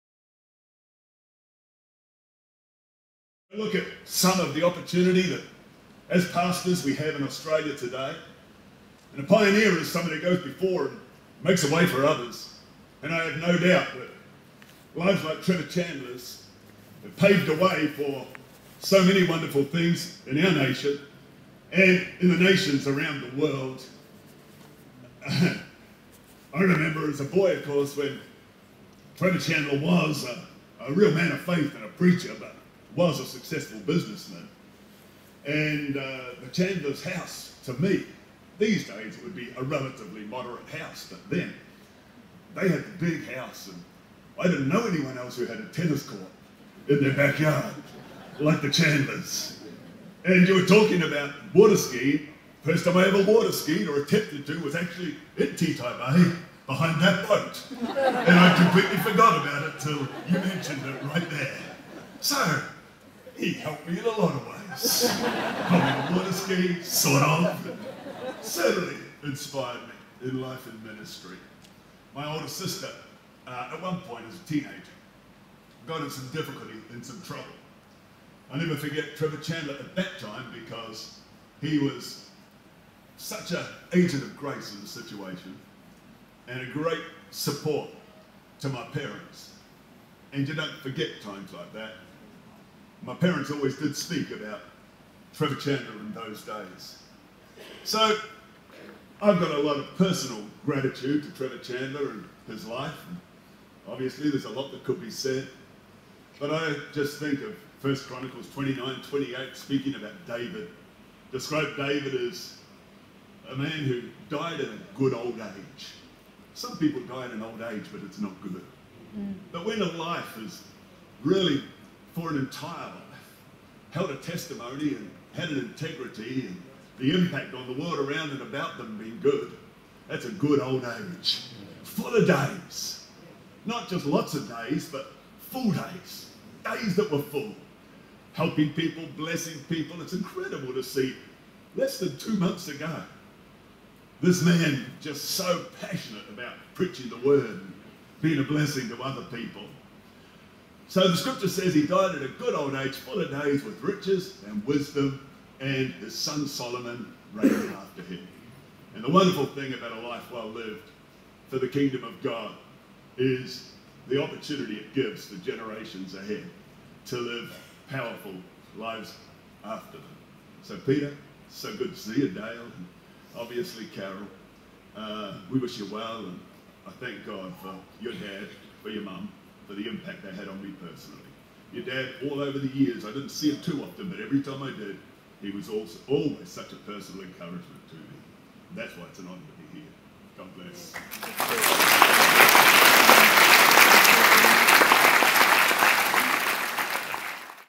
Brian C. Houston Pays Tribute To Ps Trevor Chandler
This was recorded at Catalyst Church in Ipswich Queensland where Brian Houston paid tribute to the role of Trevor Chandler in shaping him in New Zealand and the Pentecostal Church in Australia.